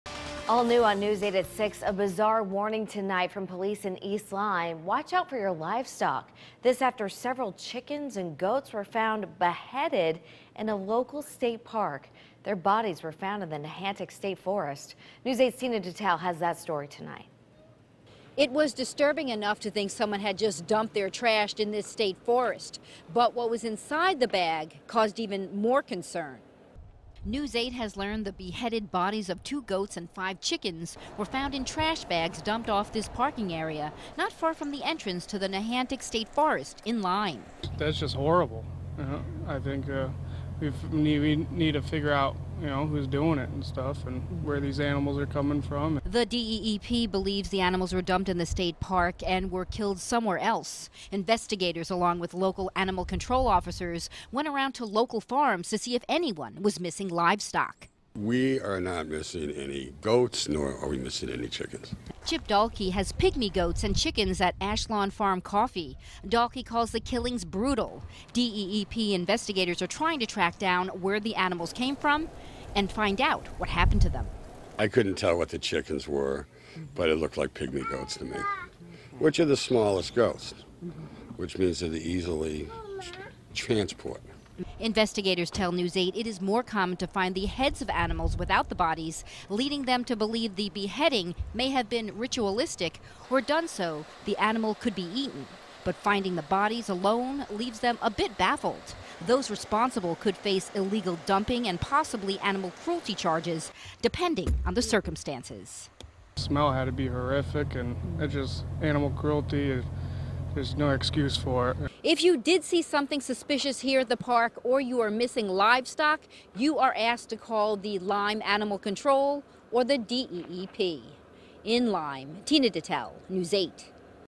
Bodies of beheaded goats, chickens found in state park
Police in East Lyme are warning residents to keep an eye on their livestock after five chickens and two goats were found beheaded inside a trash bag dumped in Nehantic State Forest.